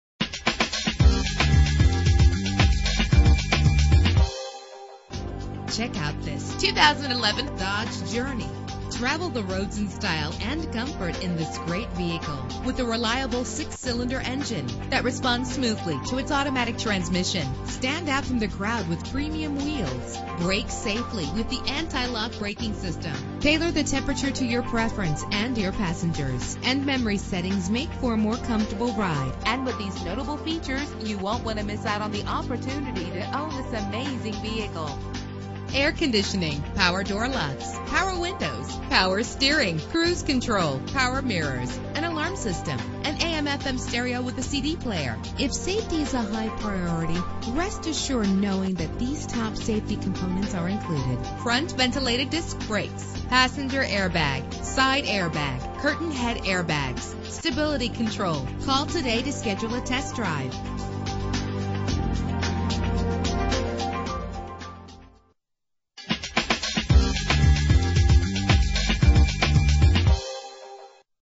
2011 Dodge Journey - Little Rock AR
Year: 2011
Make: Dodge
Model: Journey
Trim: Mainstreet
Engine: 3.6 liter 6 cylinder 24 valve
Transmission: Automatic
Color: Bright Silver Clearcoat Metallic
Mileage:
Address:
4600 South University Ave